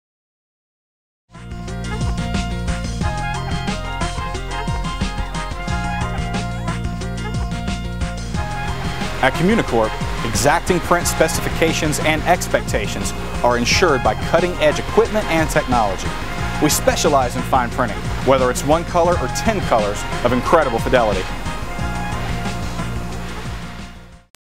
Communicorp: Conventional Print
This is a quick clip that features our conventional print assets & processes.